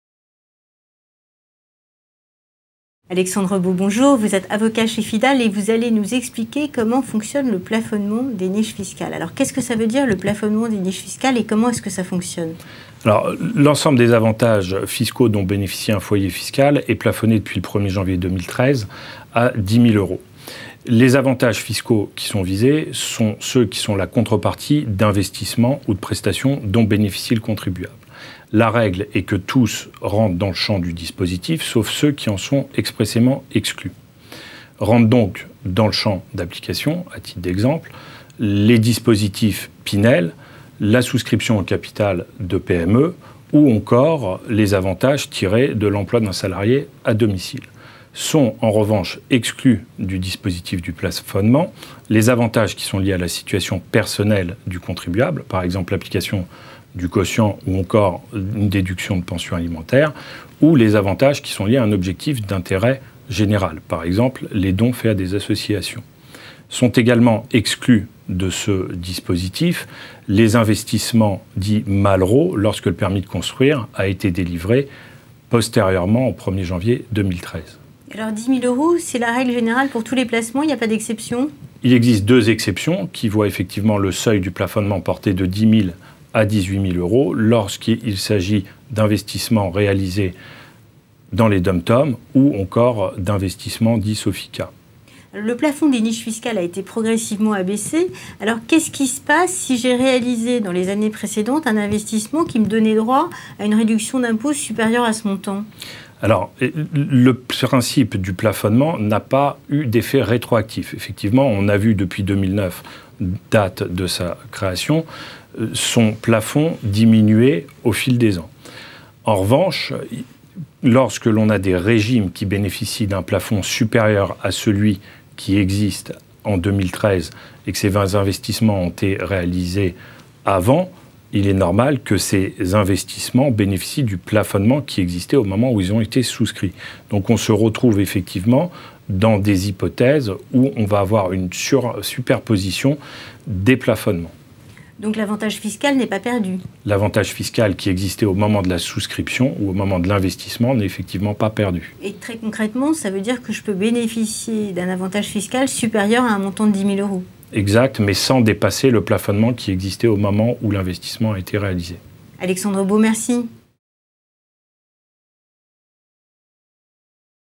Comment fonctionne le plafonnement des niches fiscales ?
L’ensemble des avantages fiscaux dont bénéficie un foyer fiscal est plafonné depuis le 1er janvier 2013 à 10.000 euros. Les explications d’Alexandre Baux, avocat chez Fidal.